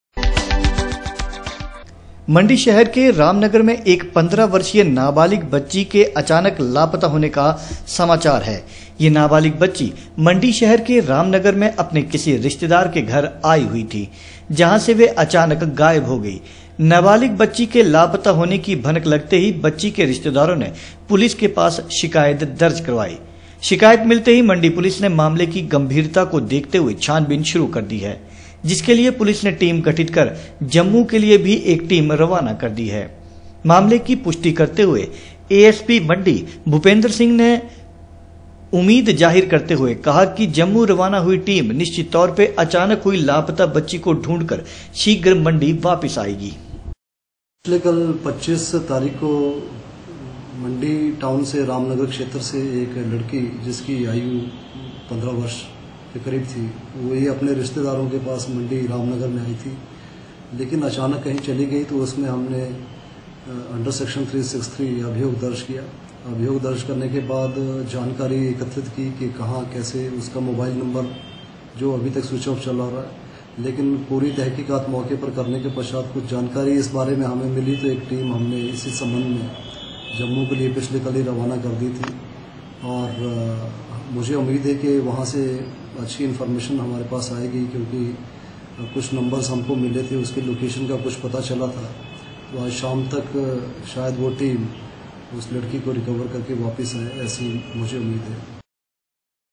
मंडी शहर के रामनगर से एक 15 वर्षीय नाबालिग बच्ची के अचानक लापता होने का समाचार
मंडी शहर के रामनगर से एक 15 वर्षीय नाबालिग बच्ची के अचानक लापता होने का समाचार है यह नाबालिग बच्ची मंडी शहर के रामनगर में अपने किसी रिश्तेदार के घर आई हुई थी जहां से वह अचानक गायब हो गई है